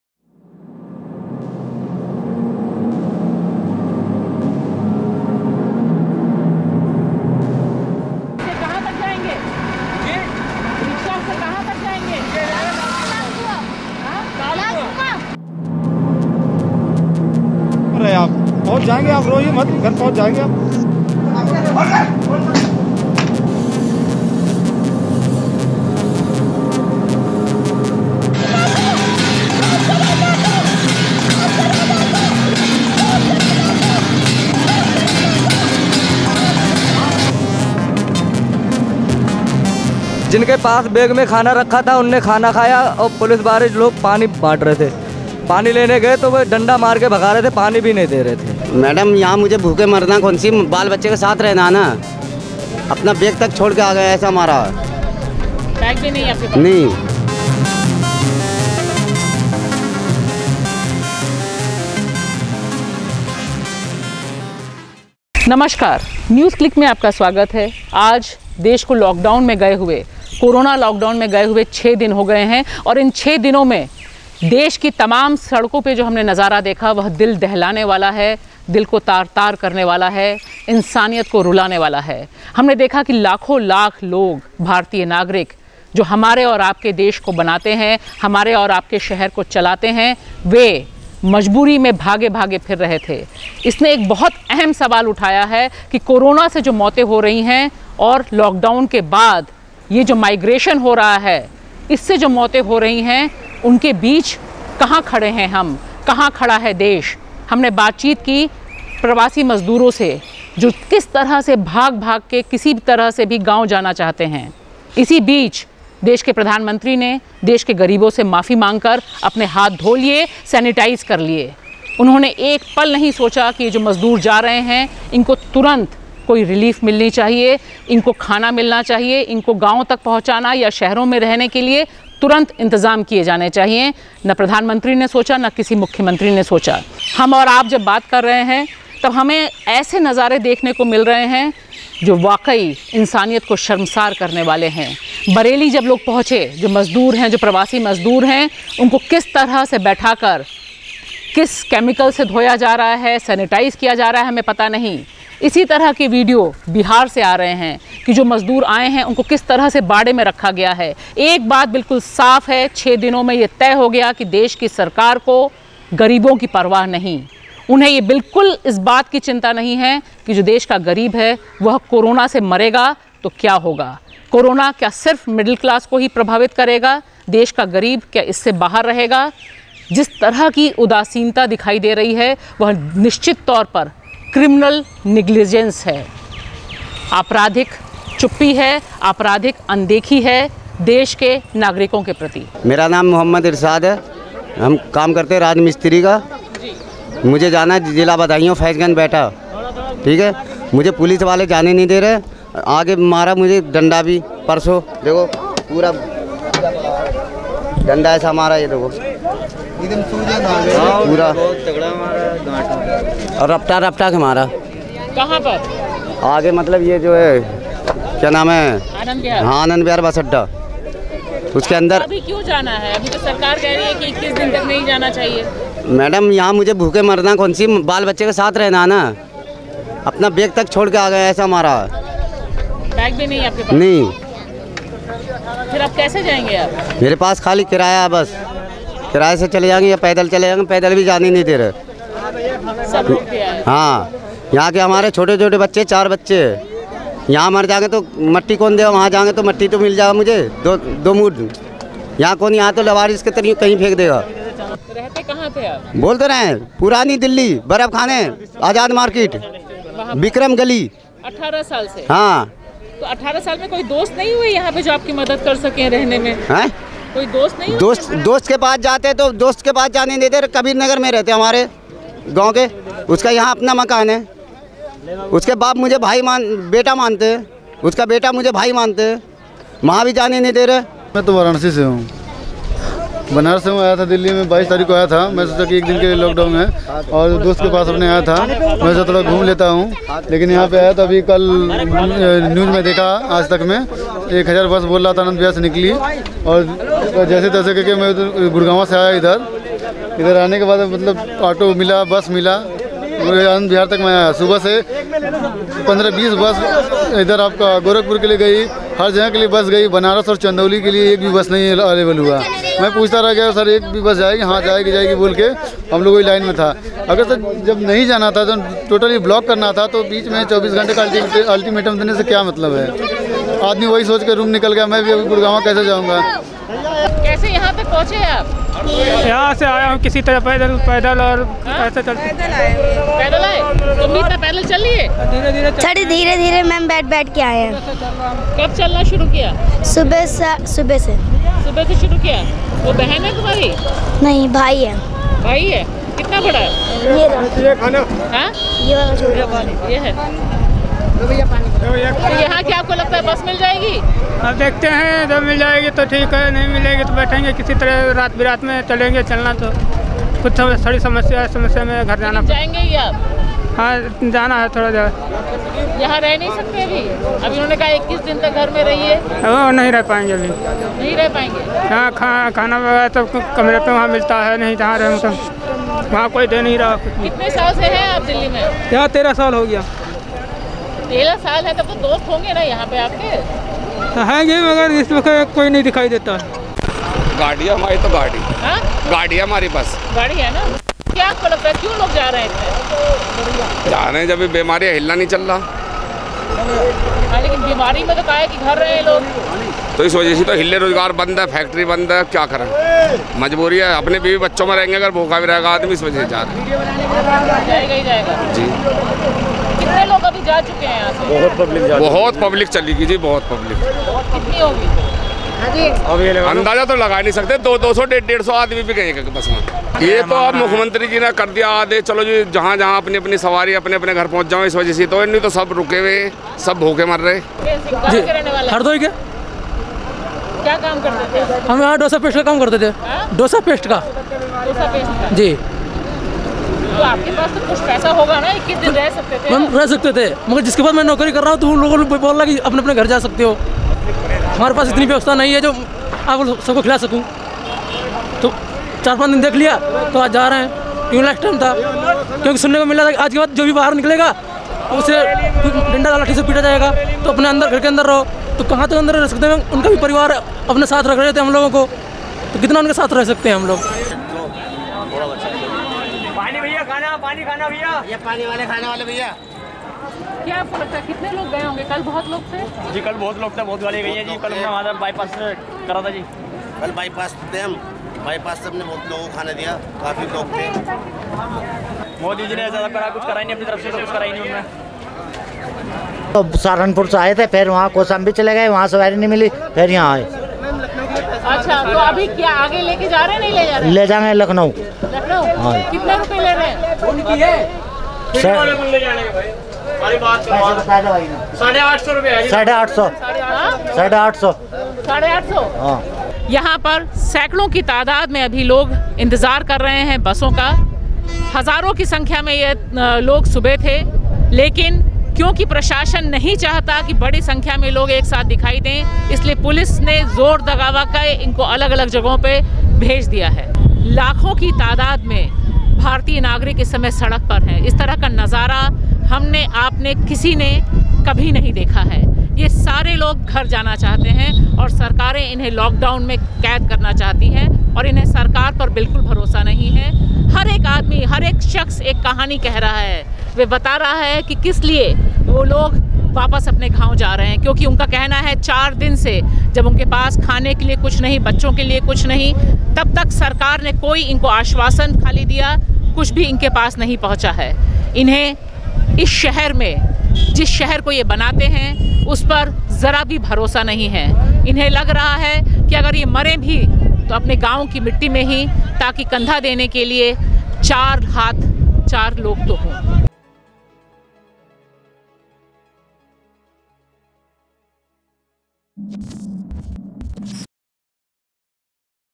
ग्राउंड रिपोर्ट: ग़रीबों को भूख से मरने को छोड़कर, Mr. PM किन्हें कोरोना से बचाएंगे?
ग्राउंड रिपोर्ट में वरिष्ठ पत्रकार भाषा सिंह ने  दिखाया कि ग़रीबों और शहरी मज़दूरों, कामगारों के बारे में बिना कोई योजना बनाए, बिना कोई वैकल्पिक इंतज़ाम किये कोरोना लॉकडॉउन की वजह से किस तरह देश के लाखों-लाख मज़दूर गांवों की तरफ पलायन करने को मजबूर हो गए। पैदल सैकड़ों किलोमीटर का सफ़र तय करते यह भारतीय नागरिक असल भारत की दर्दनाक तस्वीर पेश करते हैं। कोरोना से जंग छेड़ते हुए मोदी सरकार समेत तमाम सरकारों के दिमाग़ से असल भारत के ये लोग सिरे से गायब हैं, क्या ग़रीबों को भूख और कोरोना से मरने के लिए छोड़ दिया गया है?